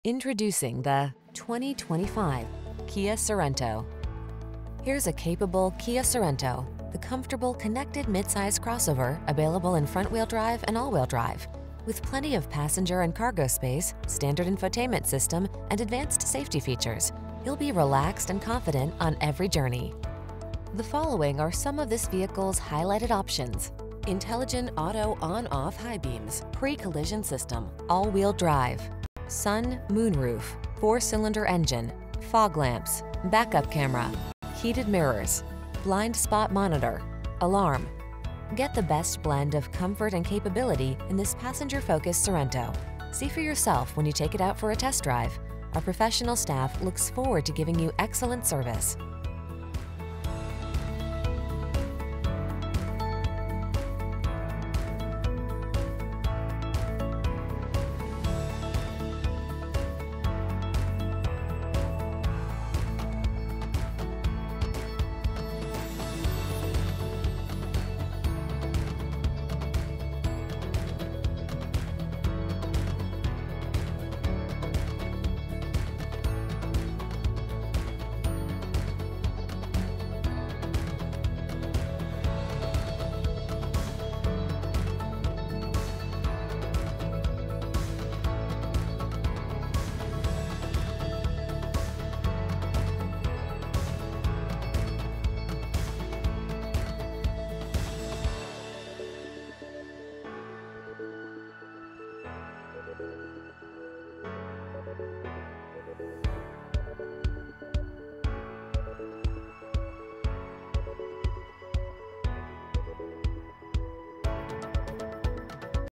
2025 Kia Sorento X-Line SX MT Billings, Bozeman, Cody WY, Sheridan WY, Butte
New 2025 Kia Sorento X-Line SX available at Kia of Billings located in 4040 King Ave W, Billings, MT, 59106 and servicing the Billings, Bozeman, Cody WY, Sheridan WY, Butte area
Please mention the stock SG319206K when you contact the dealership for this vehicle.